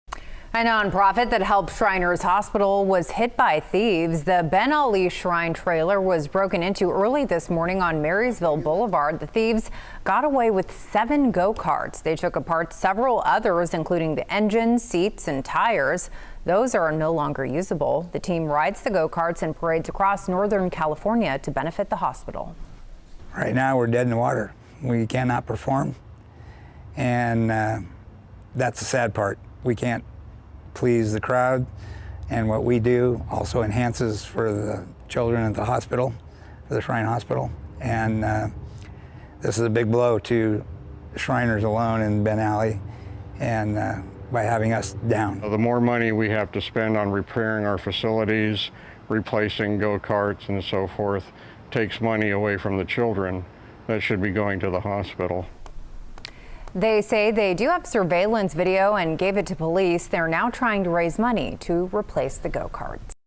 Sacramento Ben Ali Shriners' Go-Karts stolen
A nonprofit organization that helps Shriners Hospital was hit by thieves this morning.